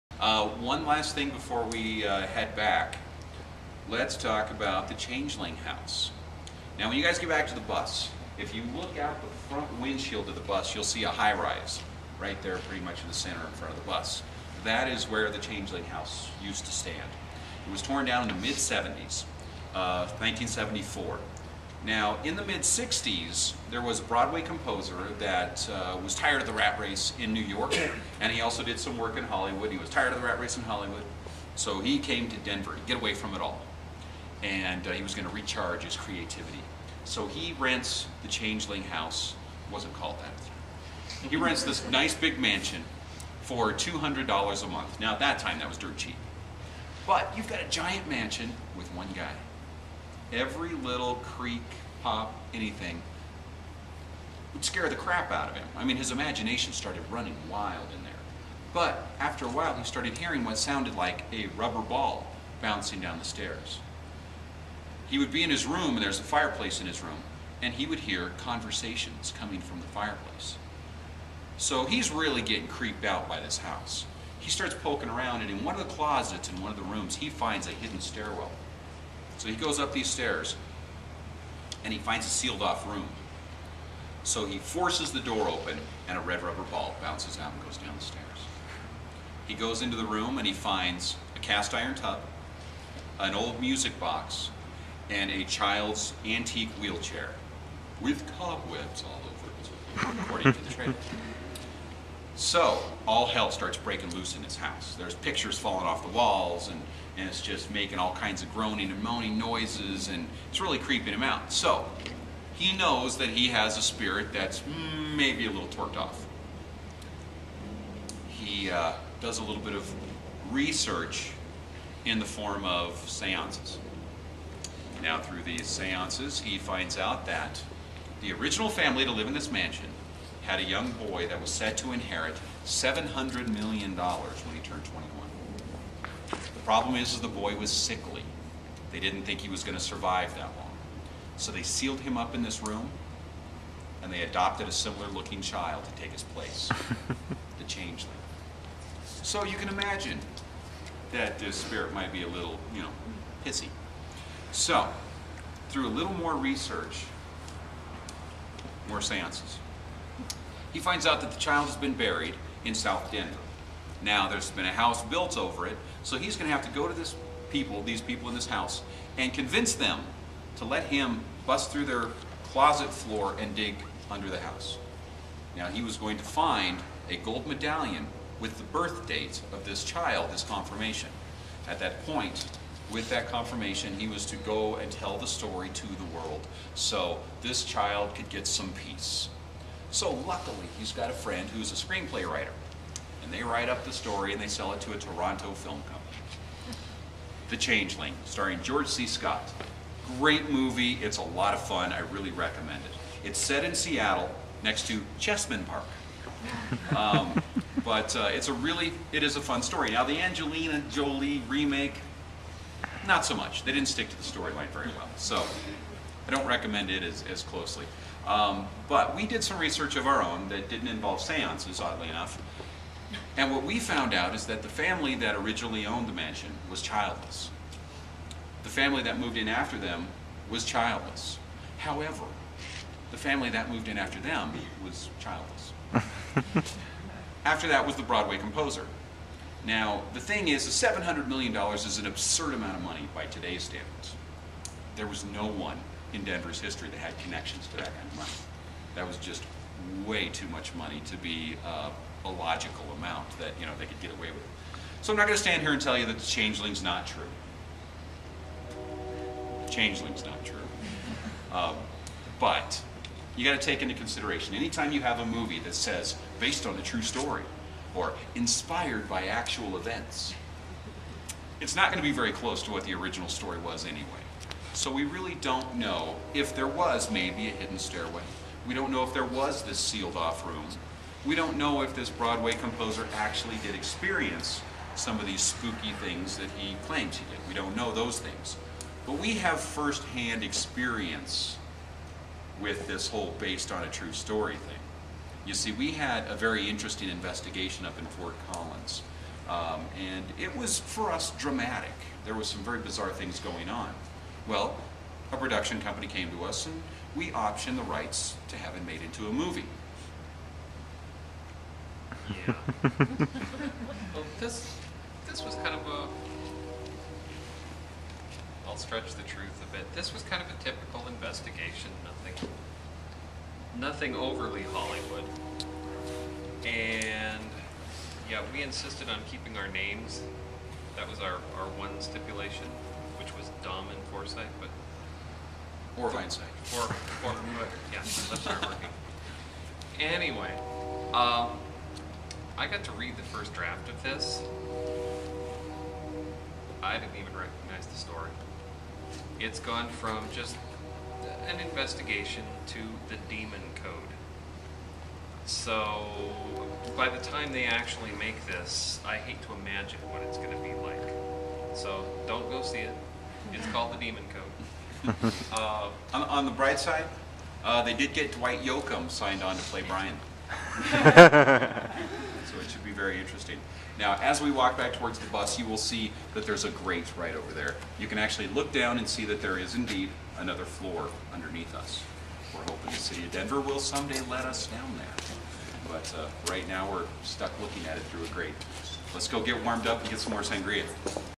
The Changeling
A brief talk about the History of Cheesman Park and it's haunted history